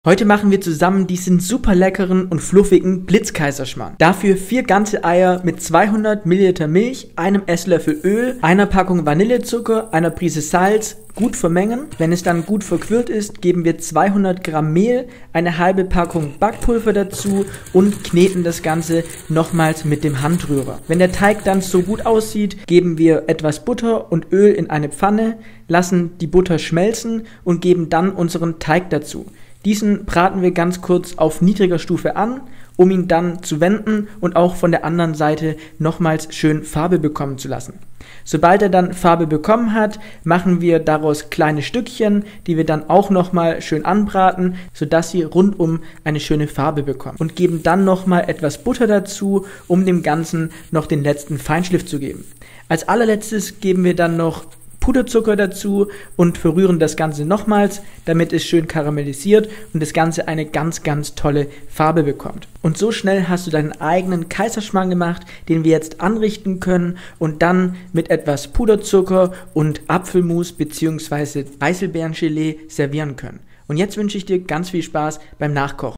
⚡️Einfacher und super leckerer Blitz Kaiserschmarrn😱😋
⚡️Super leckerer Blitz Kaiserschmarrn😱😋

Zutaten:
4 Eier
200ml Milch
1 EL Öl
1 Pck Vanillezucker
200g Weizenmehl
0,5 Pck Backpulver
Stück Butter & Öl
Puderzucker